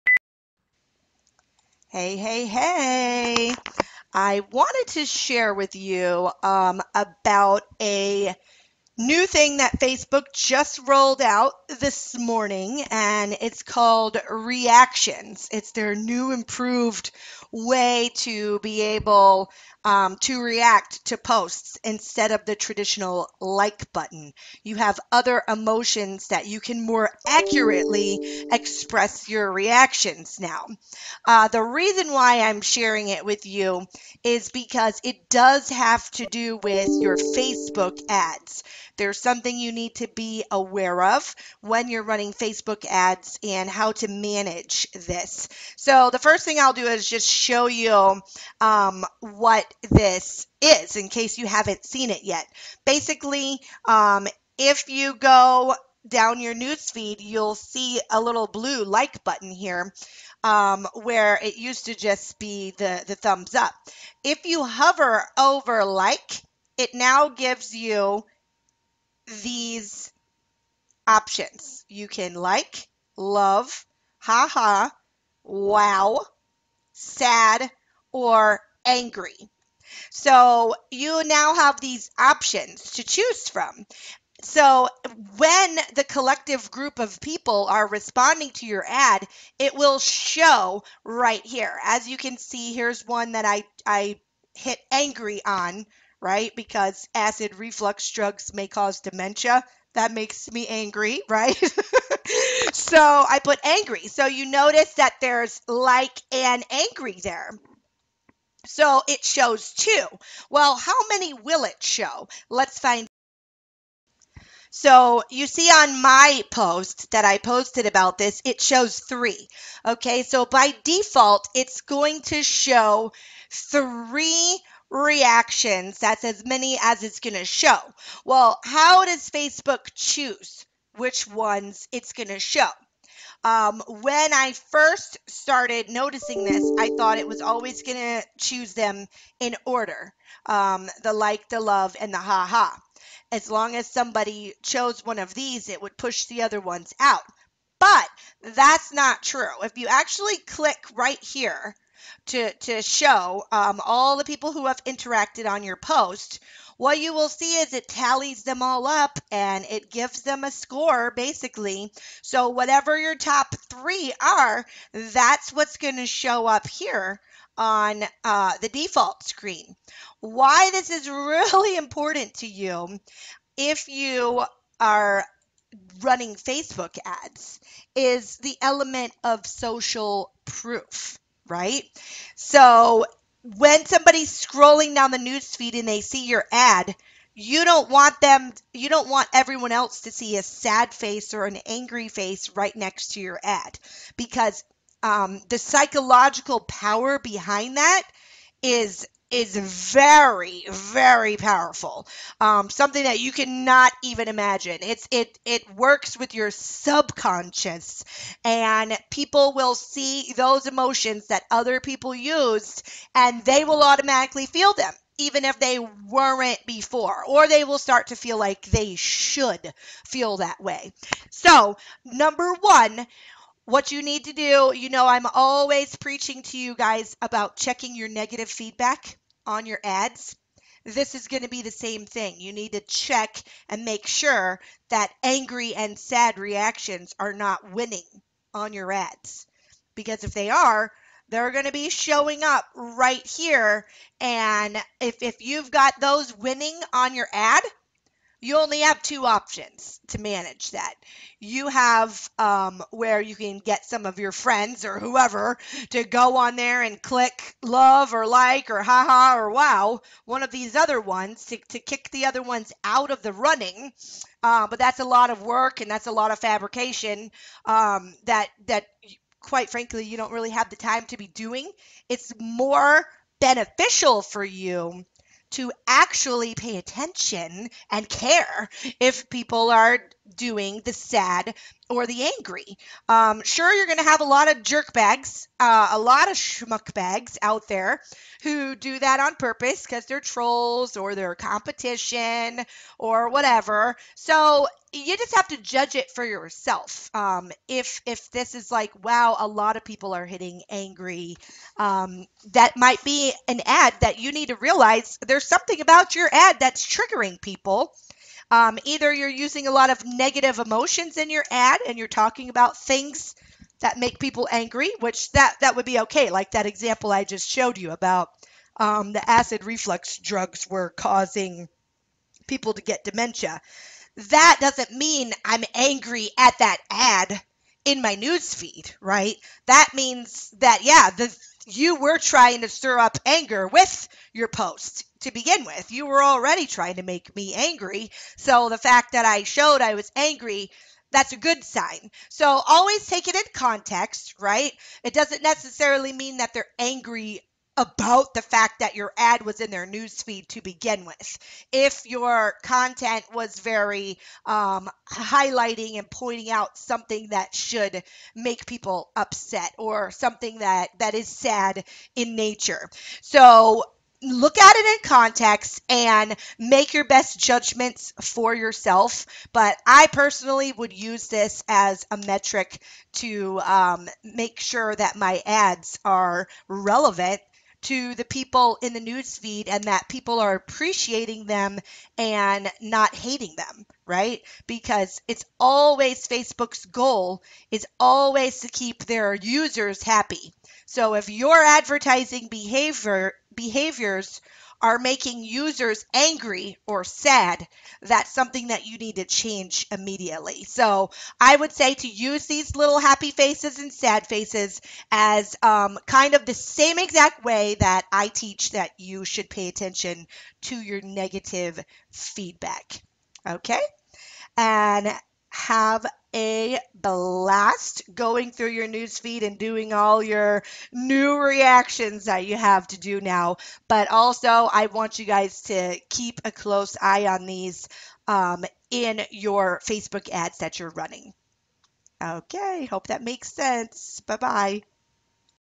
How The New Facebook Reactions Will Effect Your Facebook Ads
Learn how the new Facebook Reactions work, and why it matters to your Facebook advertising efforts. Facebook phased out the traditional like button with the new and improved reactions. Here's why you NEED TO CARE about these indicators when running Facebook Ads.

Facebook has reported that the reactions of others will NOT affect your "reach" of posts, but that does not mean you should ignore the reactions or not take them into account.

The best insight and data any marketer could ever receive is the real-life responses from real people. So yes, these Facebook reactions should matter to you, A LOT.

Join the internet's best community for folks who like to geek out on marketing